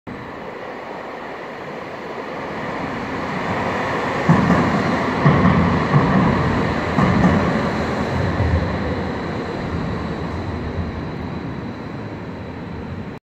RuhrBahn NF2 Linie 109 Einfahrt Rathaus Essen
In diesem Video sehen wir eine NF2 als Linie 109 am Rathaus Essen.

Aufgenommen mit dem Samsung Galaxy S7 Edge.